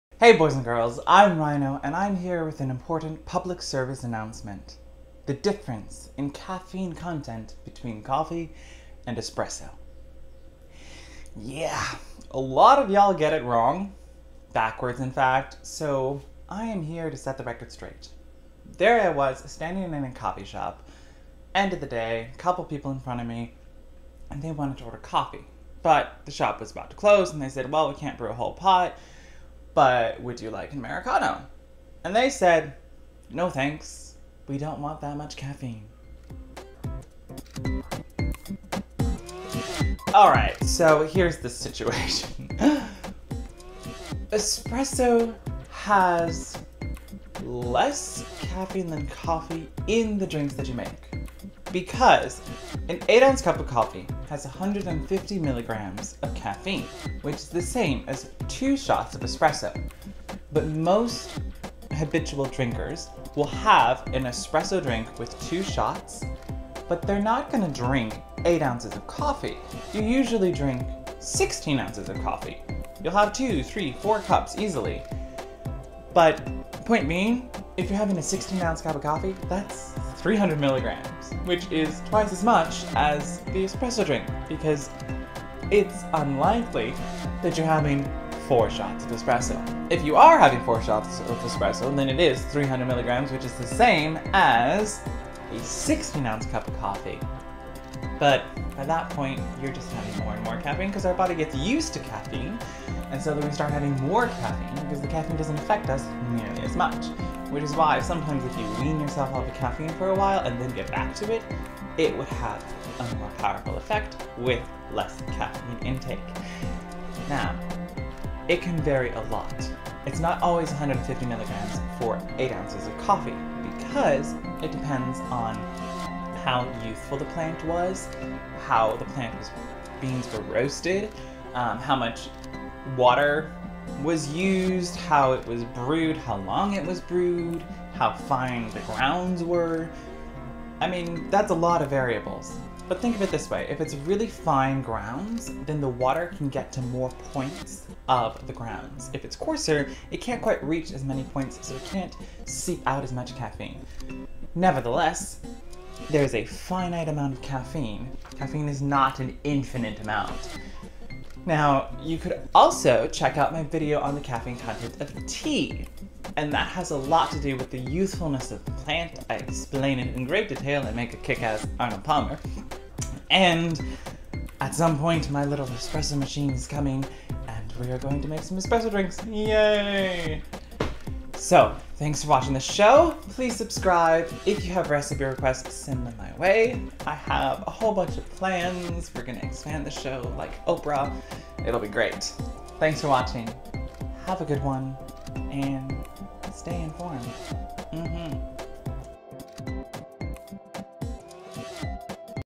Caffeine in Coffee vs Espresso! a PSA from Le Chef Paresseux Ep. 46
Which has more caffeine? Coffee or espresso? Well technically espresso are coffee beans that are brewed differently; but it all has to do with how much we are consuming. The answer may surprise you!